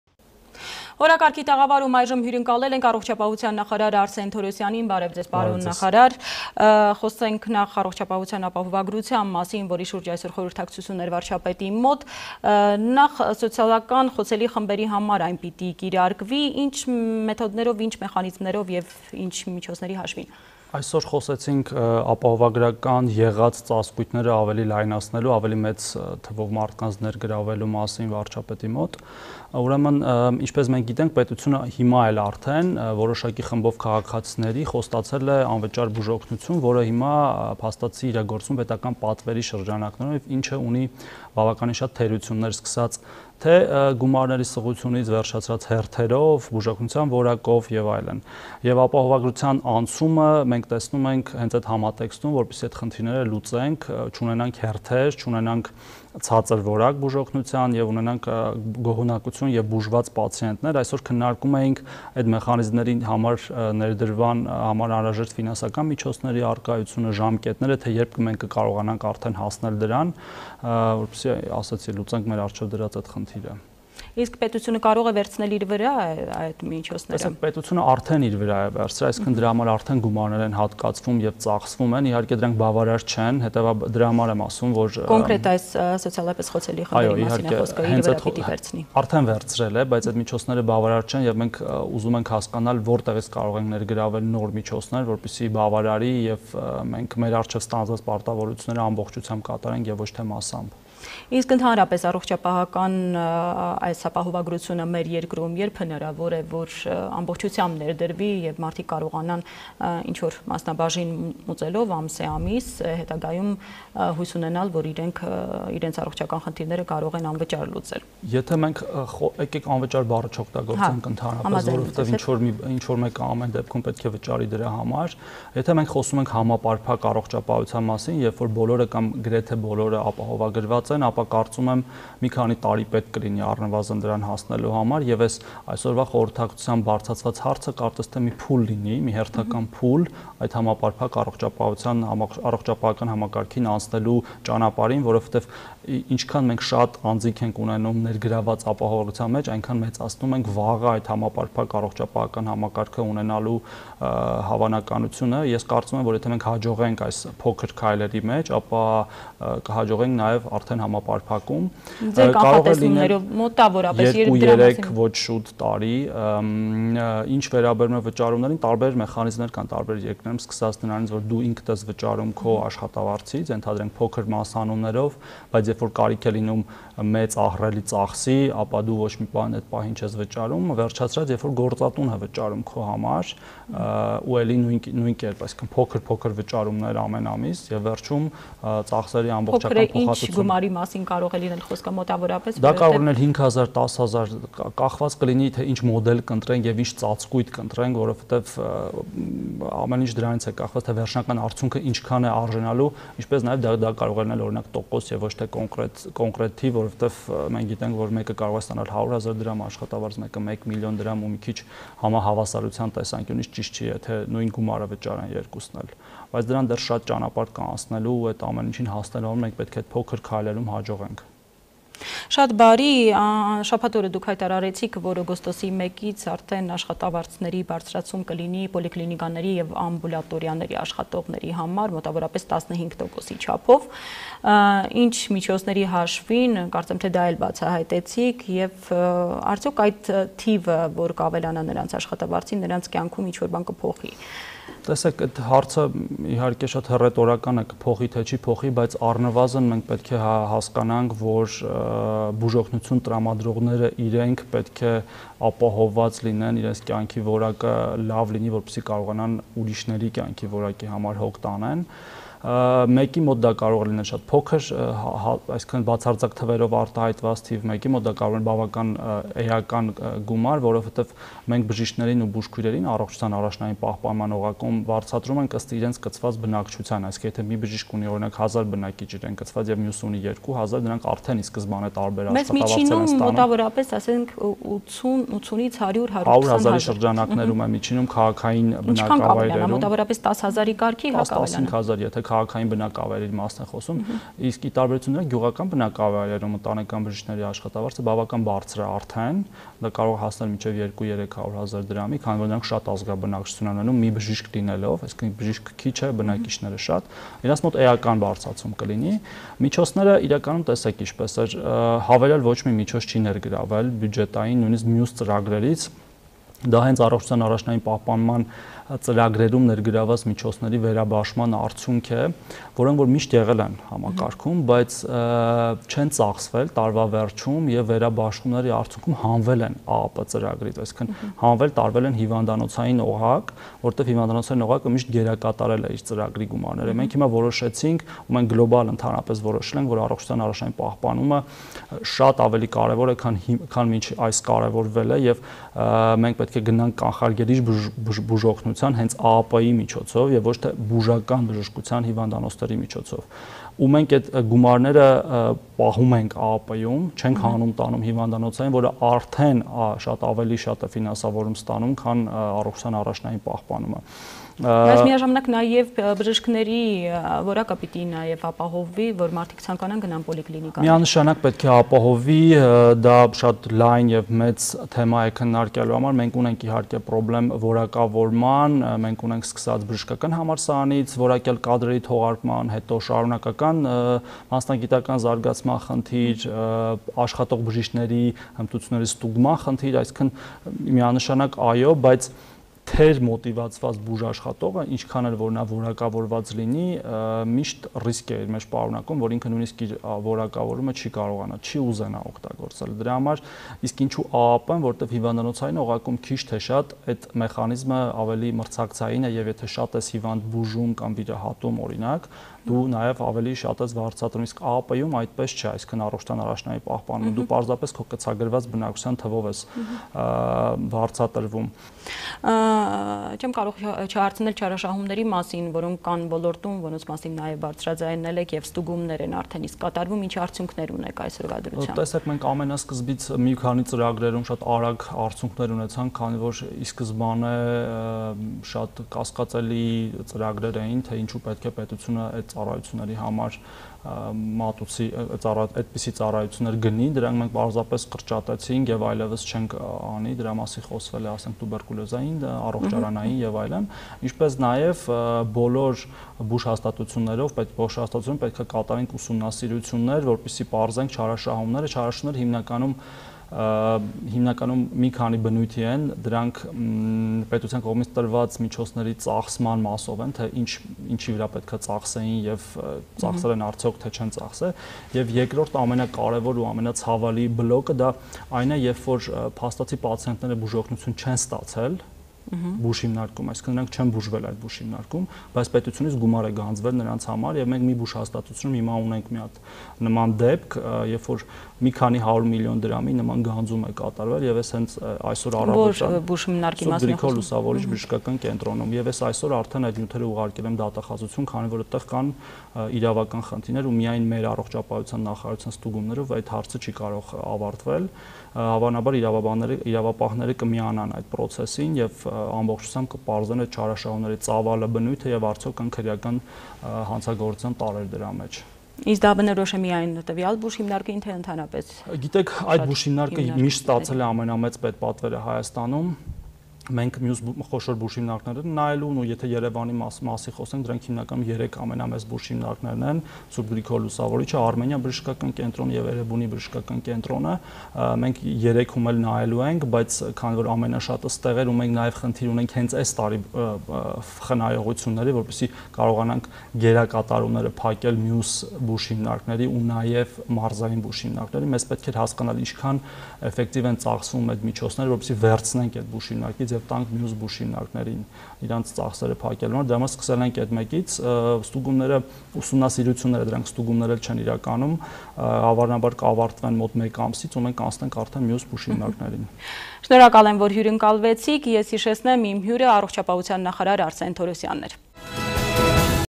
Աշխատավարձի բարձրացում, ստվերի կրճատում
Առաջնային օղակի բուժաշխատողների աշխատավարձերի բարձրացման, «Սուրբ Գրիգոր Լուսավորիչ» բժշկական կենտրոնում ընթացող ուսումնսիրությունների և ընթացիկ բարեփոխումների մասին հարցազրույց առողջապահության նախարար Արսեն Թորոսյանի հետ «Օրակարգի» տաղավարում: